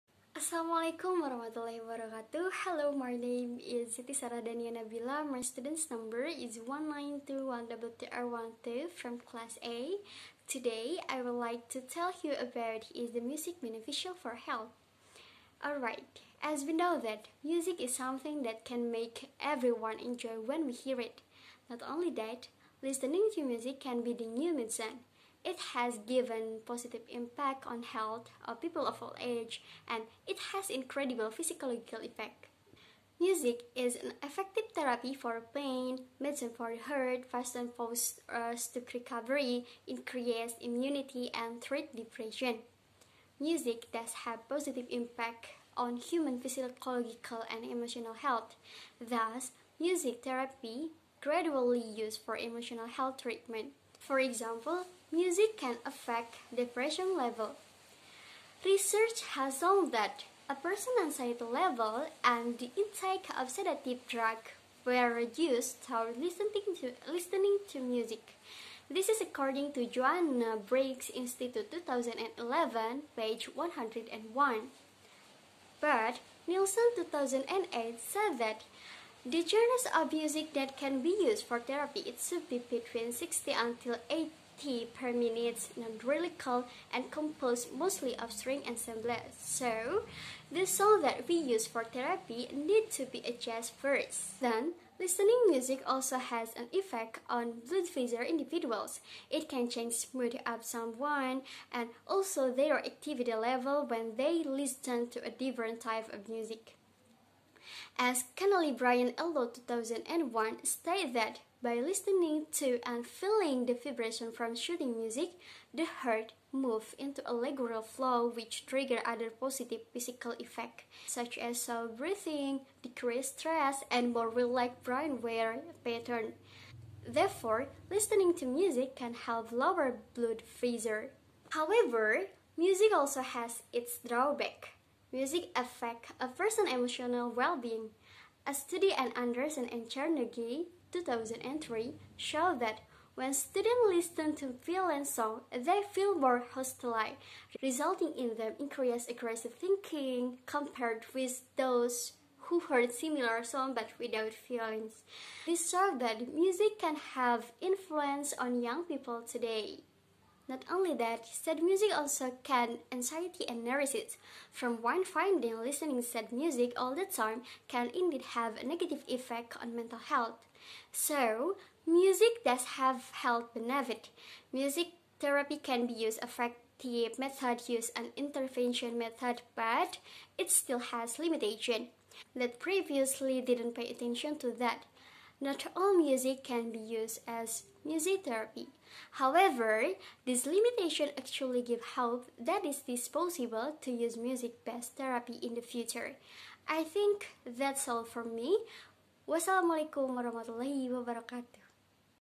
Argumentative Speech - Is music beneficial for health?
My Name is Siti Sarah Dania Nabila (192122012) from Class A.